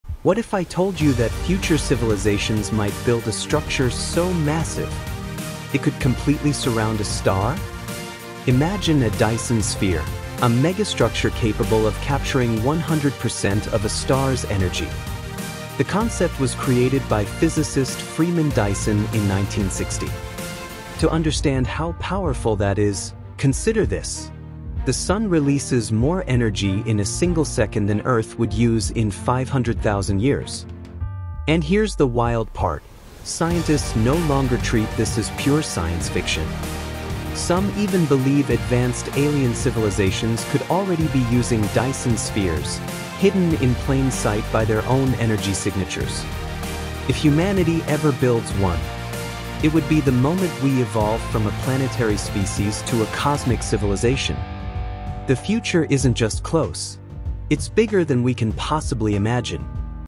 Dyson Sphere: The Mind-Blowing Mega-Structure That Could Power Humanity Forever
What if a future civilization could build a structure big enough to completely surround a star?
A Dyson Sphere isn’t science fiction anymore — it’s a real theoretical megastructure capable of capturing 100% of a star’s energy output.

This short explains how a Dyson Sphere could power humanity for hundreds of thousands of years, why scientists believe it may already exist around distant stars, and what this technology means for our future as a cosmic civilization.

If you're fascinated by space, megastructures, alien technology, or the far future of humanity, this video is for you.

Stay curious. The universe is always expanding — and so are we.